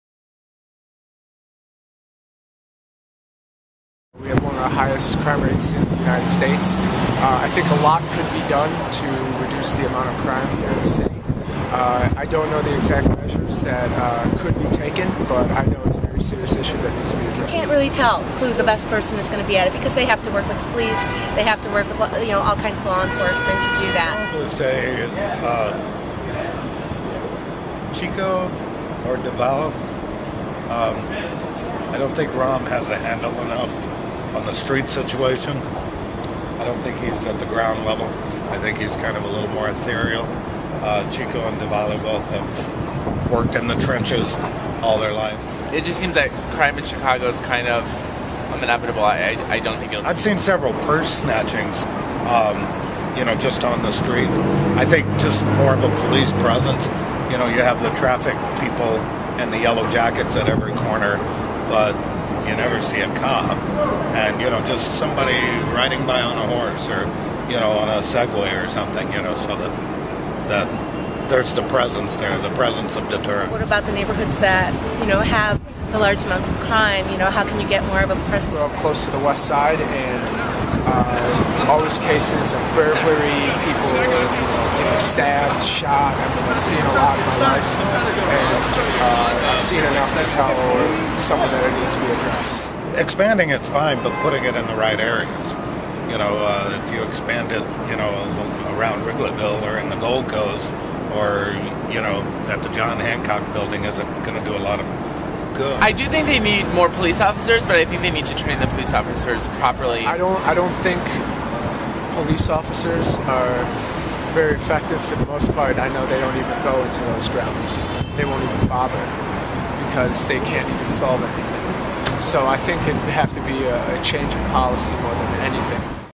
Chicago Mayoral Election Coverage
Citizens give opinions on the state of crime in Chicago and which candidate is best for the job.